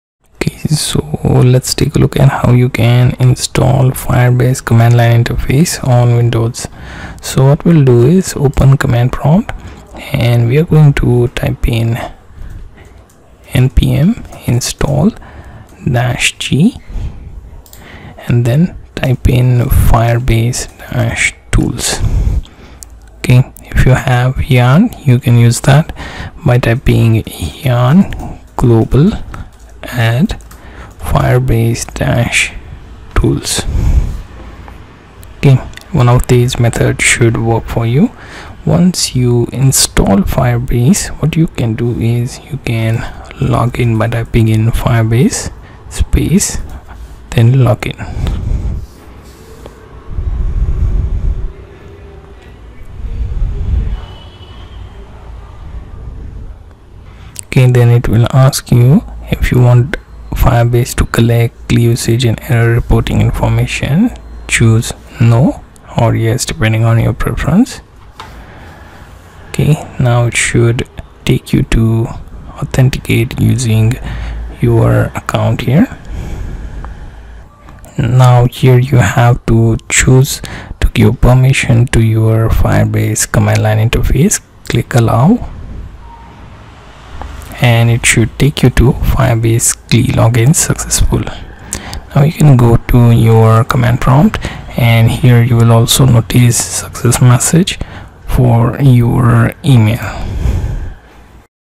Firebase CLI - How to Install FireBase CLI on Windows
Learn How to Install FireBase CLI on Windows. Install the Firebase CLI with Node on Windows desktop.